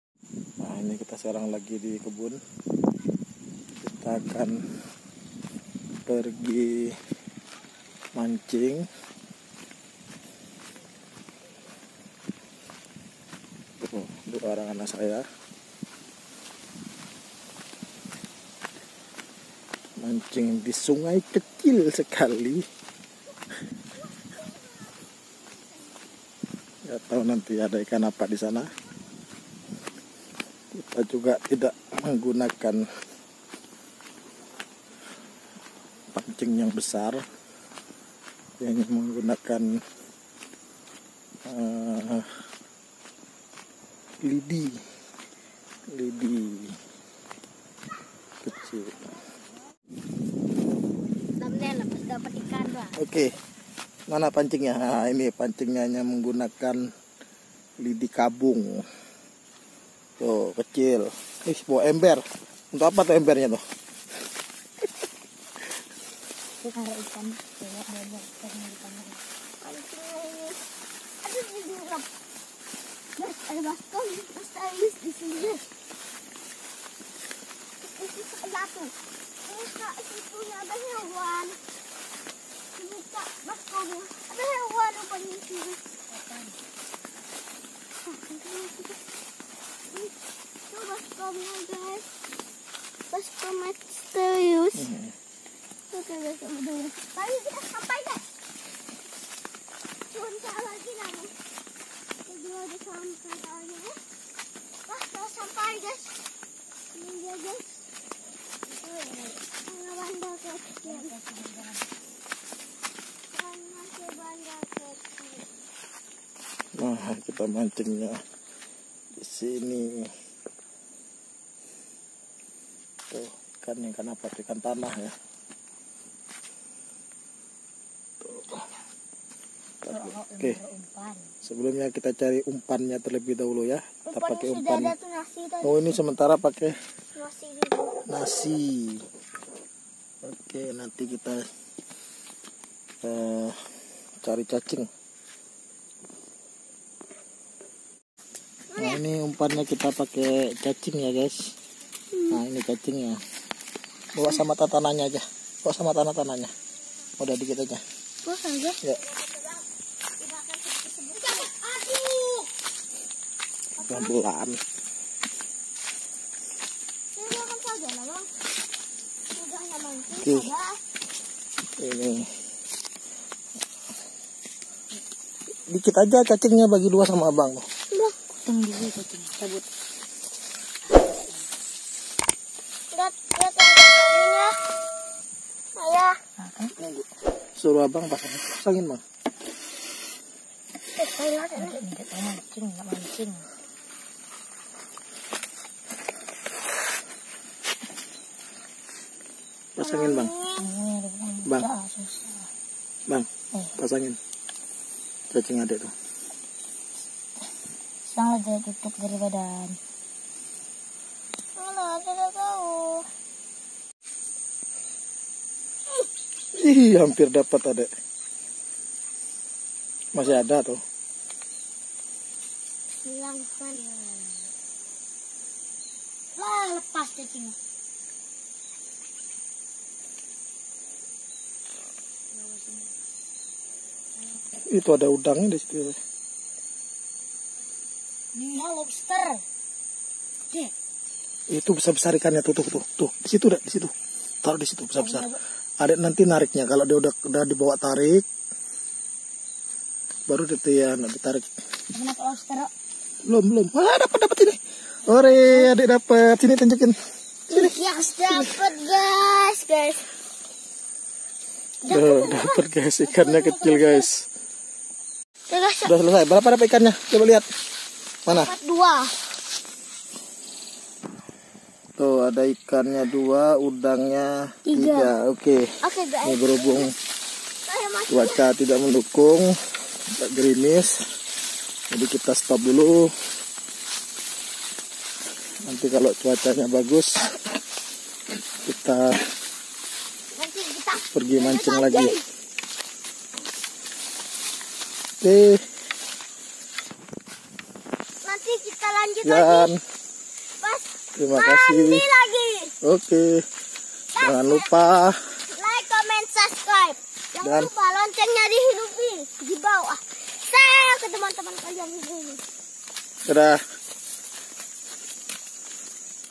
MANCING DI ALIRAN SUNGAI | STRIKE IKAN DAN UDANG KECIL
mancing di aliran sungai, strike ikam kecil.

mancing mania
strike
mancing ikan sungai
mancing udang sungai